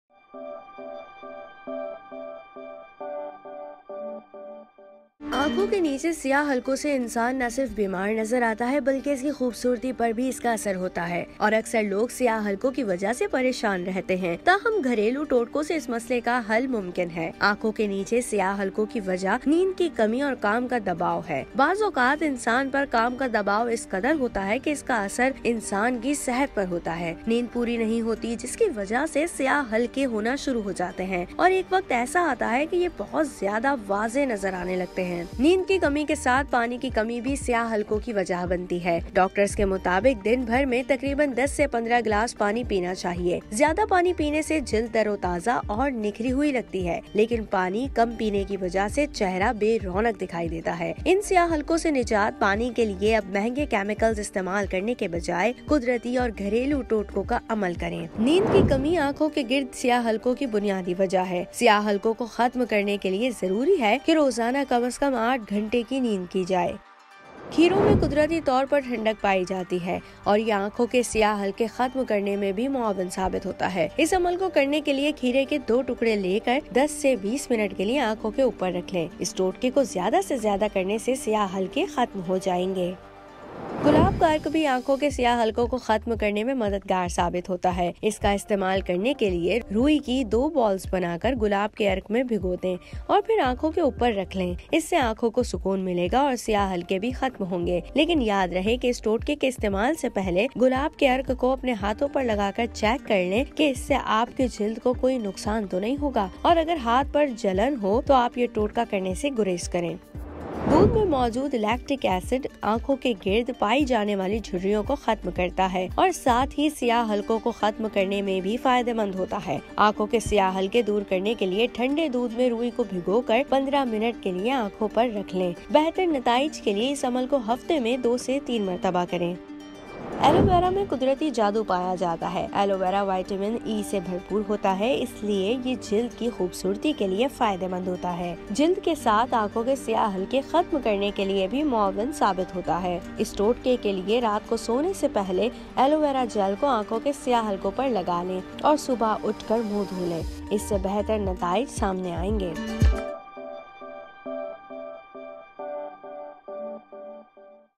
Home Remedies for Dark Circles Under Eyes | Say Goodbye to Tired Eyes!
Tired of those persistent dark circles under your eyes? Look no further! In this video, we share some of the best home remedies to help reduce and eliminate dark circles naturally. Learn about simple ingredients and easy methods that you can try at home to achieve brighter, more refreshed eyes. Whether it's cucumber slices, almond oil, or tea bags, we've got you covered with tips and tricks that really work.

 / themodernhospital